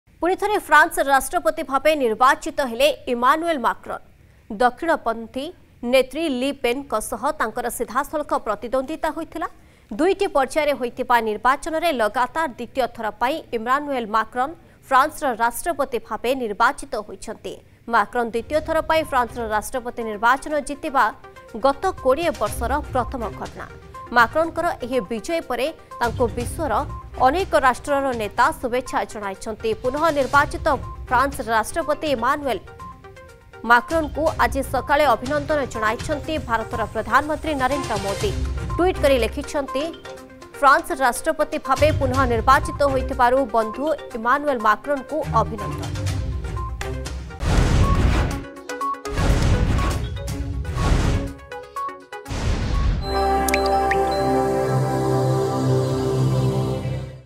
Emmanuel Macron Re-Elected President of France, defeating Far-Right Candidate Marine Le Pen
Emmanuel Macron, the incumbent President of France, has been re-elected for a second term, defeating far-right candidate Marine Le Pen.